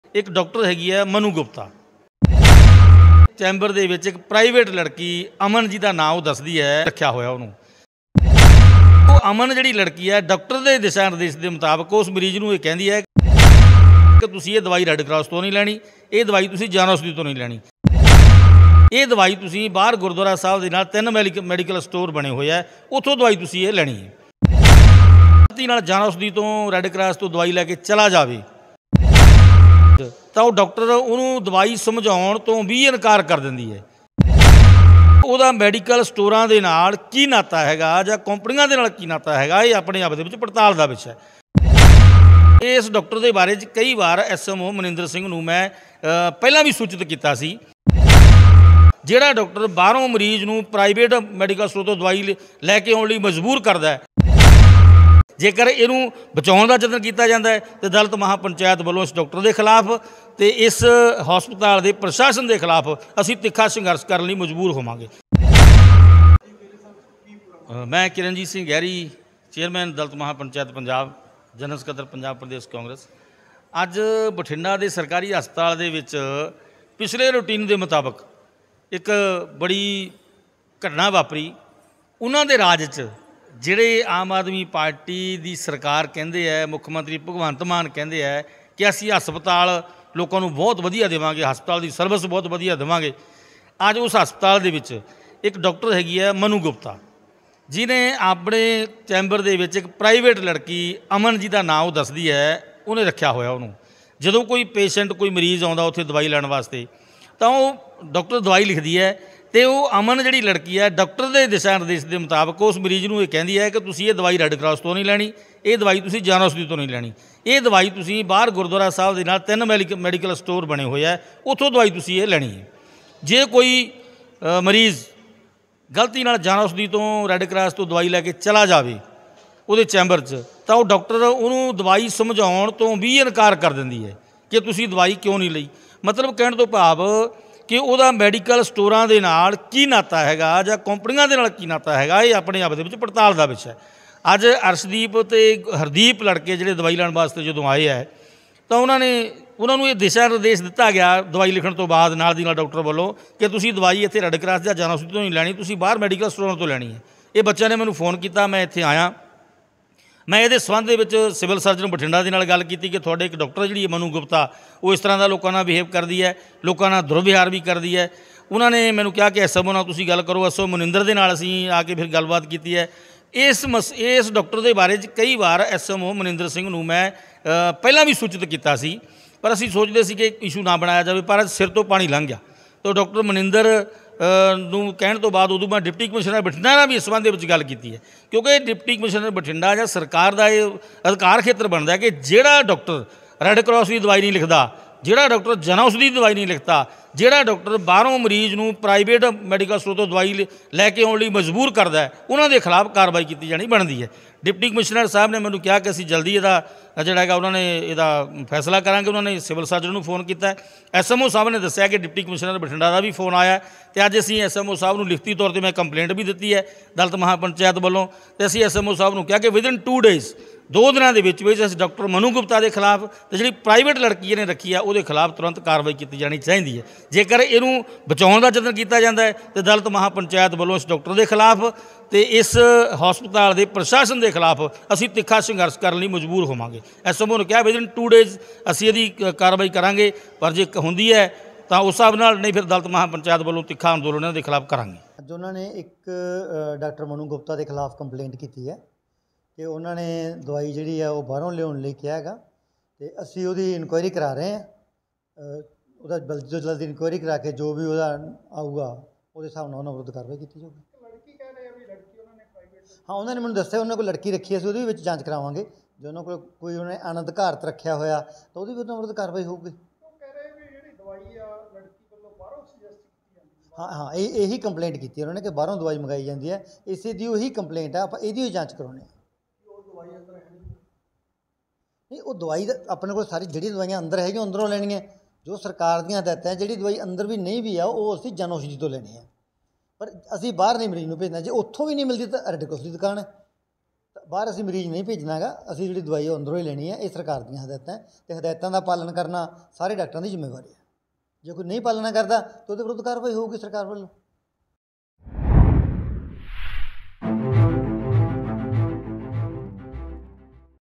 ਡਾਕਟਰ ਮਨੂੰ ਗੁਪਤਾ ਦੀ ਕਿਰਨਜੀਤ ਸਿੰਘ ਗਹਿਰੀ ਵੱਲੋਂ ਕੀਤੀ ਸ਼ਿਕਾਇਤ।ਮਰੀਜ਼ਾਂ ਤੋਂ ਮੰਗਵਾਈਆਂ ਜਾਂਦੀਆਂ ਬਾਹਰੋਂ ਦਵਾਈਆਂ
ਡਾਕਟਰ ਮਨੂੰ ਗੁਪਤਾ ਦੀ ਕਿਰਨਜੀਤ ਸਿੰਘ ਗਹਿਰੀ ਵੱਲੋਂ ਕੀਤੀ ਸ਼ਿਕਾਇਤ। ਕਿਹਾ ਮਰੀਜ਼ਾਂ ਤੋਂ ਮੰਗਵਾਈਆਂ ਜਾਂਦੀਆਂ ਬਾਹਰੋਂ ਦਵਾਈਆਂ। SMO ਨੇ ਦਿੱਤੇ ਜਾਂਚ ਦੇ ਆਦੇਸ਼।SMObathinda